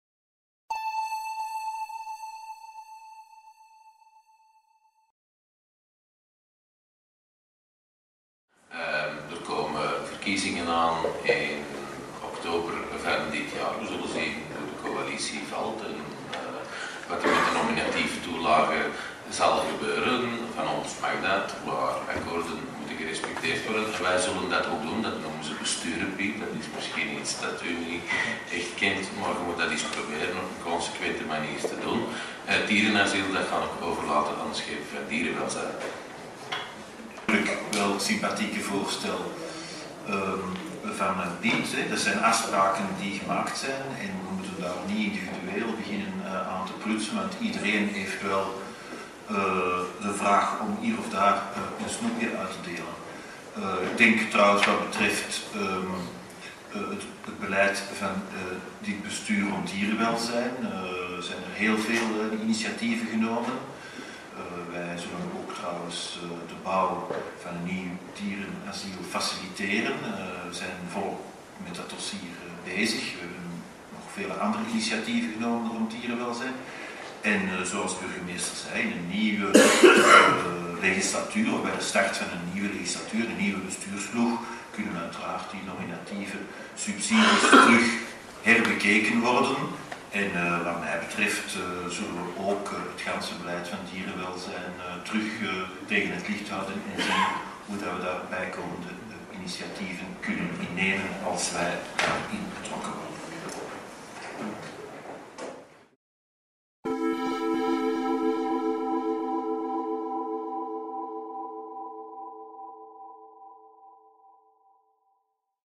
Nominatieve Toelagen
Naast een verdubbeling van de toelage voor Vlaamse Initiatieven vraagt het VB bij monde van Piet Bouciqué ook een verdubbeling van de toelage voor het Dierenasiel gezien de moeilijke omstandigheden waarin dit asiel met hoofdzakelijk vrijwilligers momenteel moet werken.
De eerste vraag wordt afgeketst door burgemeester De Veuster (N-VA), de tweede door schepen Valkeniers (VLD). Copyright Belhamel Schoten